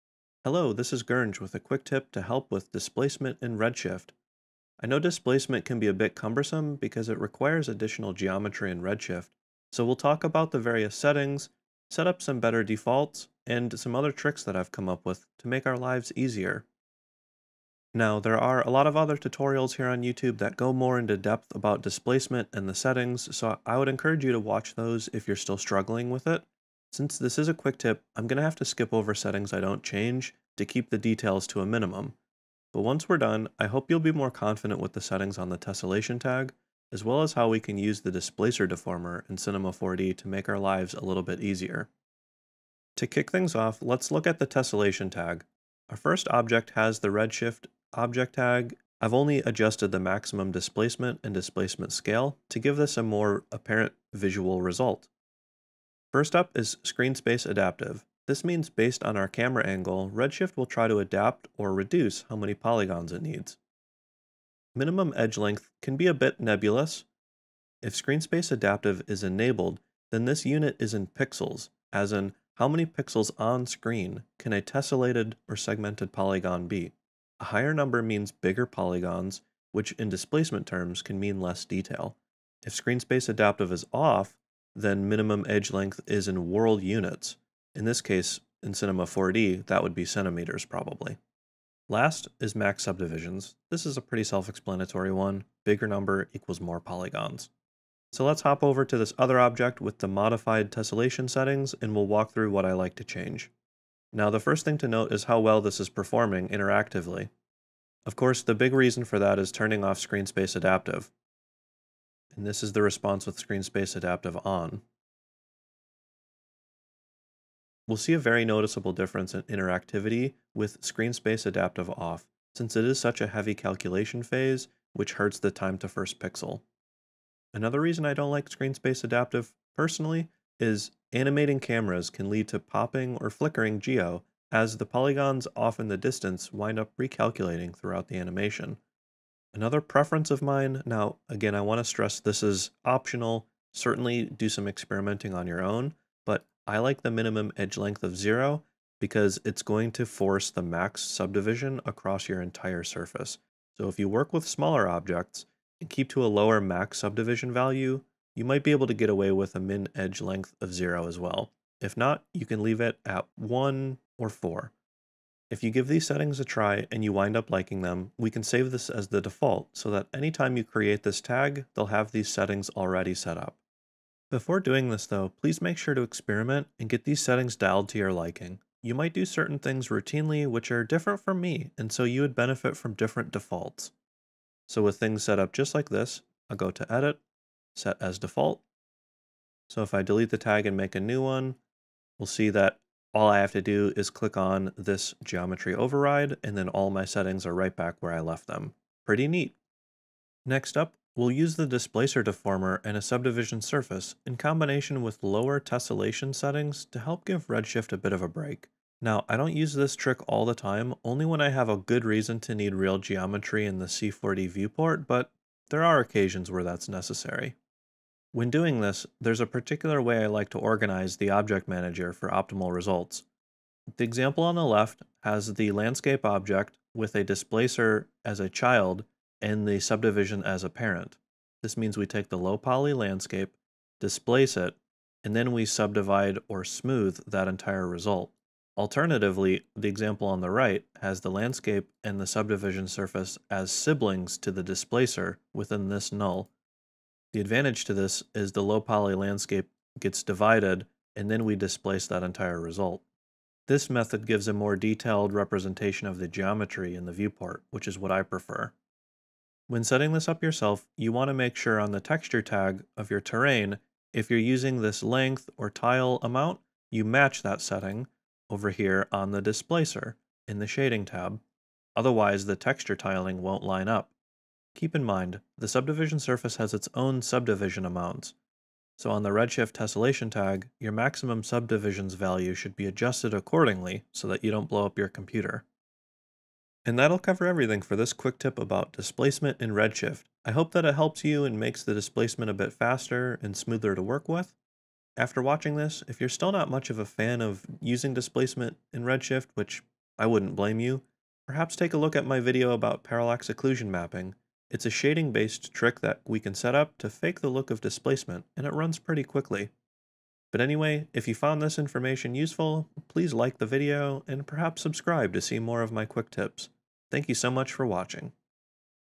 Gernge Quick Tips - Redshift Displacement Tips | Cinema 4D Redshift
This quick tip is a walkthrough of the Tessellation Tag settings in Redshift, which are most commonly used to generate the polygons necessary for Displacement to work. There are a handful of settings I prefer to change, so that interactivity performance is smoother, and those settings can easily be saved as the defaults.

Additionally, I cover my method to use the Cinema 4D Displacer Deformer which can help generate some of the necessary geometry in the viewport. This can be useful in certain situations, so I include how I like setting it up.

The Tessellation Tag settings should apply to Redshift in other DCCs as well.

00:00 Introduction
00:42 Tessellation Tag Overview
01:38 My Recommended Tessellation Settings
02:45 Saving Improved Settings As Defaults
03:30 Using The Cinema 4D Displacer Deformer
04:46 Conclusion